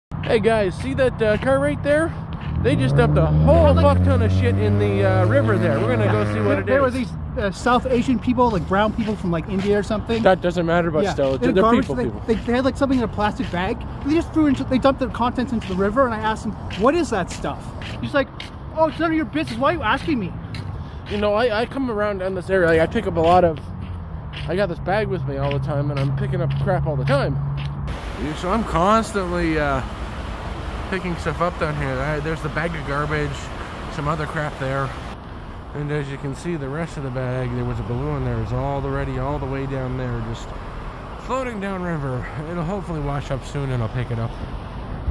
chasing down peoplemdumpjng garbage in the river
caught some.peoplw.dumping garbage theres context after the clip of me and a random stranger chatting them down/ catching them dump